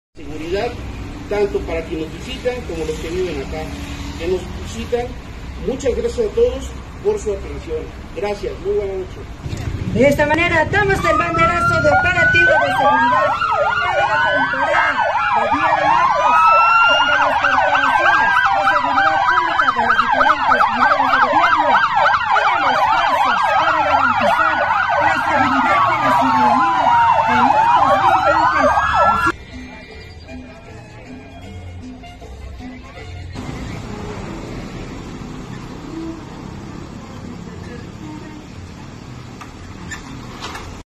En Atlixco inicia el operativo de seguridad "Todos Santos 2024" a cargo de la Guardia Nacional
En Atlixco, inicia el operativo de seguridad "Todos Santos 2024" a cargo de la Guardia Nacional, Policía Estatal, Policía Municipal, Protección Civil y Patrulla Naranja, para brindar vigilancia a vendedores y visitantes por la celebración del Día de Muertos.